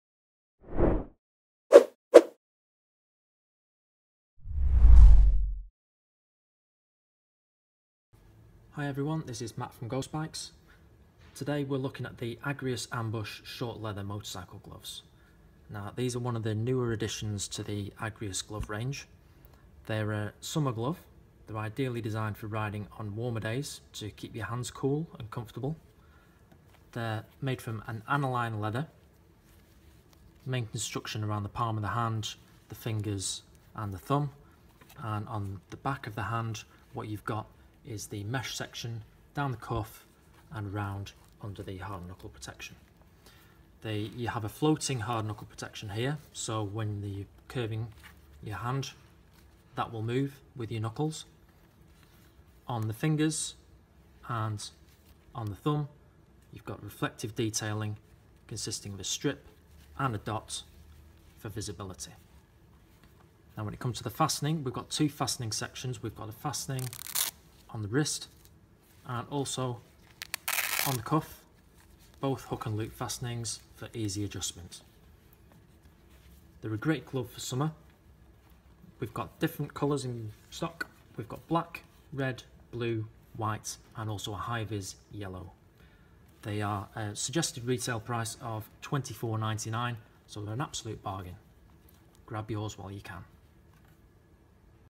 Review of Agrius Ambush Short Leather Motorcycle Gloves - Ghostbikes
A review of the Agrius Ambush Short Leather Motorbike Gloves

The Agrius Ambush Motorbike Gloves are the newest addition to the Agrius range. Constructed from high quality Analine leather blended with a breathable textile mesh these gloves are designed ensure your hands stay cool and comfortable on even the hottest day. The stylish design combined with the floating hard knuckle and reflective finger Detailing allow for additional protection and visibility without effecting comfort or flexibility make this the ideal glove or all riders.

Key Features / Points:

Analine Leather Construction
Floating Hard Knuckle
Perforated Textile Mesh
Stretch Panel On Fingers
Reflective Finger detailing